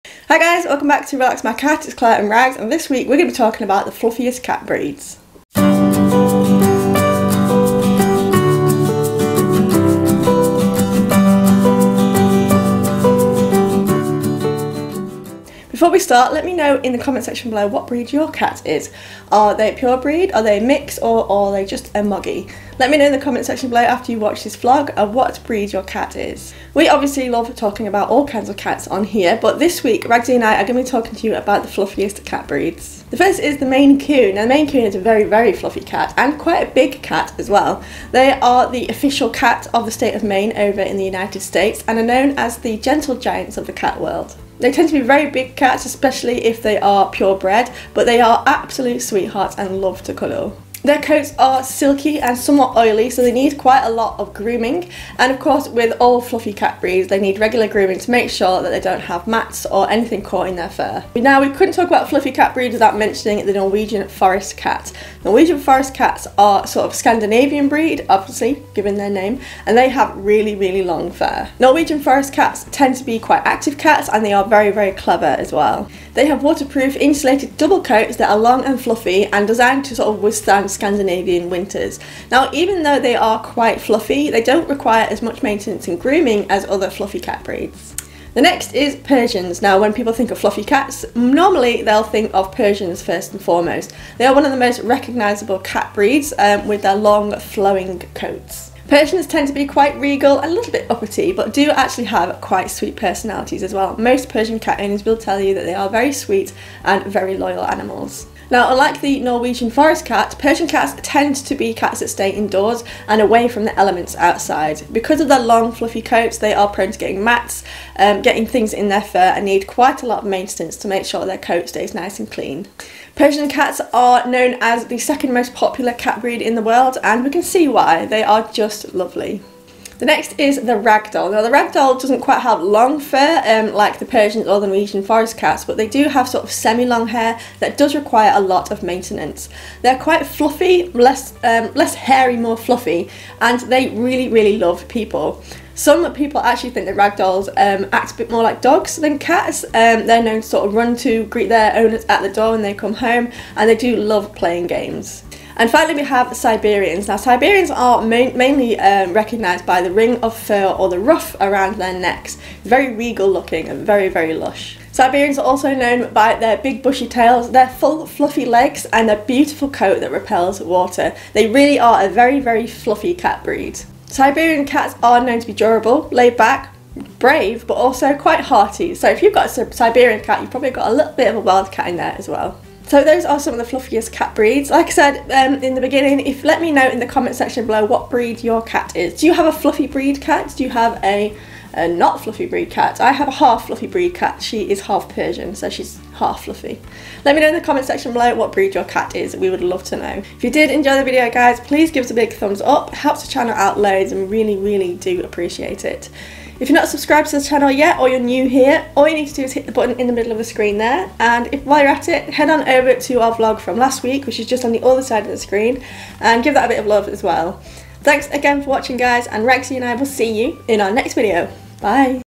TOP 5 Fluffiest Cat Breeds EVER | You Won't Believe the Cuteness!
We at Relax My Cat love talking about cats - all kinds of cats! From the fluffy to the bald; the long to the short!

This week Claire and Rags are back sharing with you the top 5 fluffiest cat breeds!